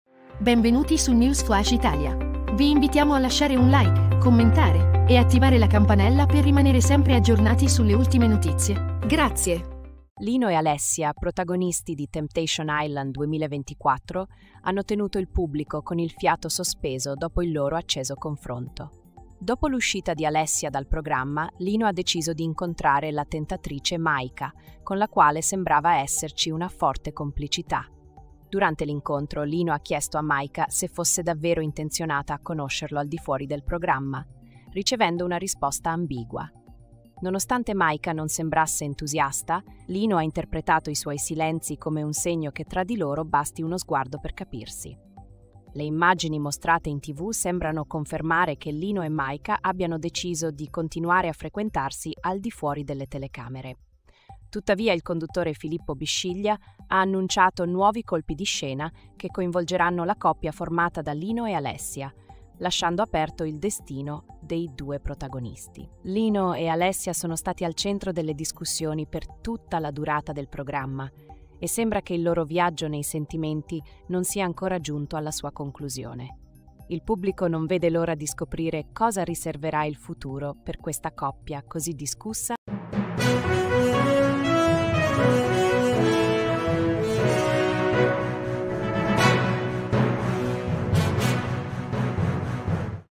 SCOPRIAMO IL DESTINO DI LINO E MAIKA DOPO TEMPTATION ISLAND 2024
Lino e Alessia, protagonisti di Temptation Island 2024, hanno tenuto il pubblico con il fiato sospeso dopo il loro acceso confronto. Dopo l'uscita di Alessia dal programma, Lino ha deciso di incontrare la tentatrice Maika, con la quale sembrava esserci una forte complicità.

Durante l'incontro, Lino ha chiesto a Maika se fosse davvero intenzionata a conoscerlo al di fuori del programma, ricevendo una risposta ambigua. Nonostante Maika non sembrasse entusiasta, Lino ha interpretato i suoi silenzi come un segno che tra di loro basti uno sguardo per capirsi.

Le immagini mostrate in tv sembrano confermare che Lino e Maika abbiano deciso di continuare a frequentarsi al di fuori delle telecamere. Tuttavia, il conduttore Filippo Bisciglia ha annunciato nuovi colpi di scena che coinvolgeranno la coppia formata da Lino e Alessia, lasciando aperto il destino dei due protagonisti.

Lino e Alessia sono stati al centro delle discussioni per tutta la durata del programma, e sembra che il loro viaggio nei sentimenti non sia ancora giunto alla sua conclusione. Il pubblico non vede l'ora di scoprire cosa riserverà il futuro per questa coppia così discussa e appassionante.